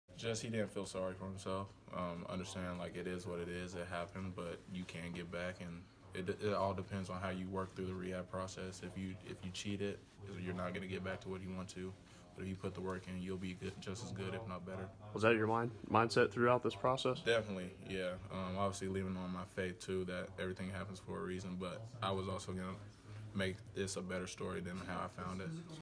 Nick Basquine leaned on Michael Crabtree blueprint in rehab
This video is about Nick Basquine leaned on Michael Crabtree blueprint in rehab